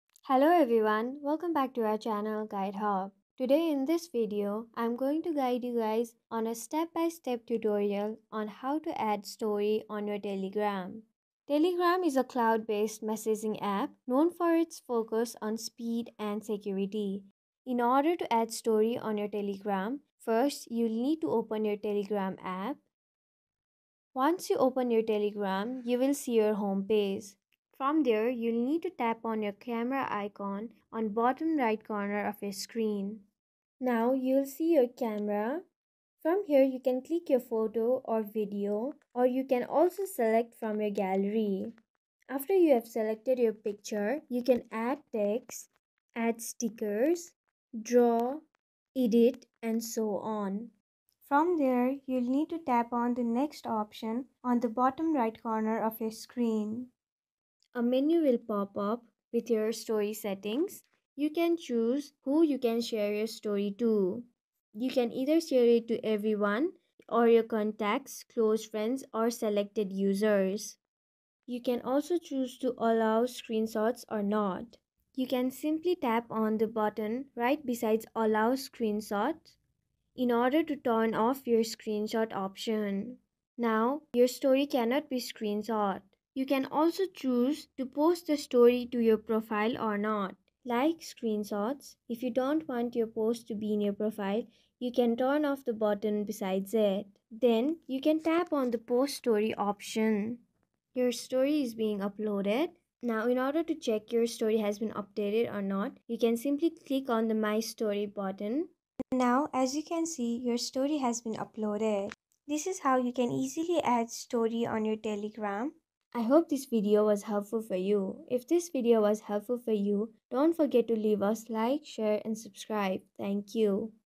How To Add Story On Telegram App 2024 | Share Your Moments With Telegram Contacts
"How To Add Story On Telegram App 2024 | Share Your Moments With Telegram Contacts" - A Tutorial Video by Guide Hub.

Want to share fleeting moments with your Telegram contacts? Master Telegram Stories in minutes with this comprehensive guide! Learn how to create captivating stories, add text, stickers, and even polls!  This video covers everything you need to know about telegram stories.